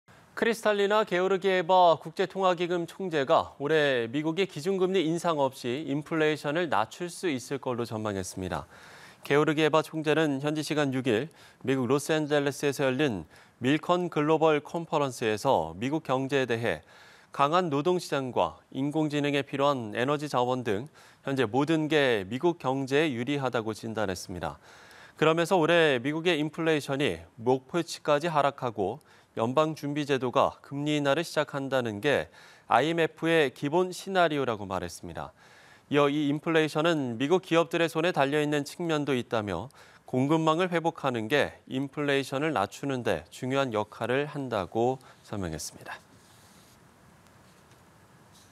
IMF 총재 "미국, 올해 인플레 잡고 금리 인하할 것" / 연합뉴스TV (YonhapnewsTV)
IMF 총재 "미국, 올해 인플레 잡고 금리 인하할 것"

크리스탈리나 게오르기에바 국제통화기금(IMF) 총재가 올해 미국이 기준금리 인상 없이 인플레이션을 낮출 수 있을 것으로 전망했습니다.

게오르기에바 총재는 현지시간 6일 미국 로스앤젤레스에서 열린 밀컨 글로벌 콘퍼런스에서 미국 경제에 대해 "강한 노동시장과 인공지능에 필요한 에너지 자원 등 현재 모든 것이 미국 경제에 유리하다"고 진단했습니다.

그러면서 올해 미국의 인플레이션이 목표치까지 하락하고 연방준비제도가 금리 인하를 시작한다는 것이 IMF의 기본 시나리오라고 말했습니다.

이어 "이 인플레이션은 미국 기업들의 손에 달린 측면도 있다"며 "공급망을 회복하는 것이 인플레이션을 낮추는 데 중요한 역할을 한다"고 설명했습니다.